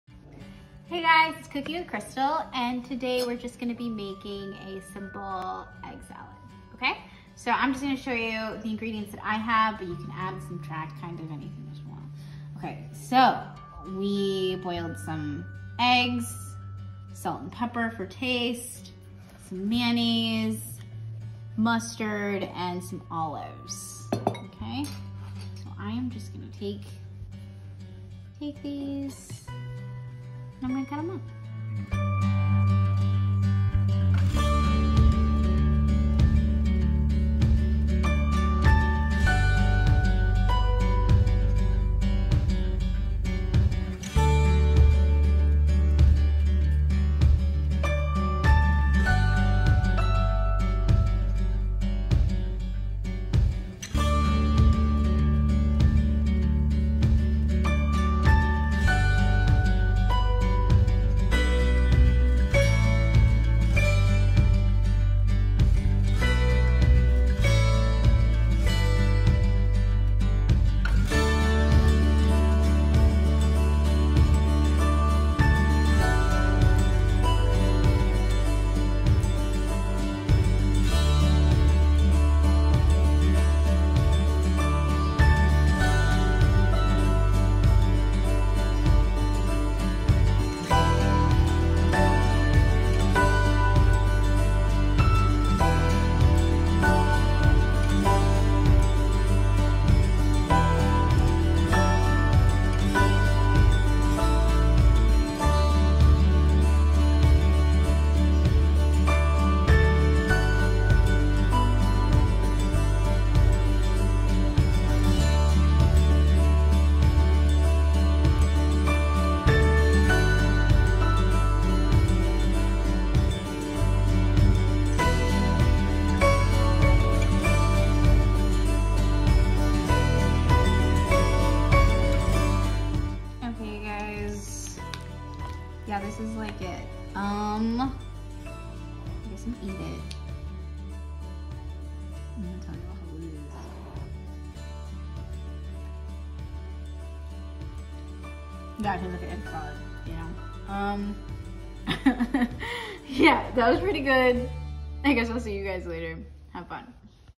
Cooking With Krystal: Egg Salad
Go crazy! there is no recipe! But we used:
olives
sweet relish
paprika
salt and pepper
low fat mayonnaise
mustard
and of course some hardboiled eggs!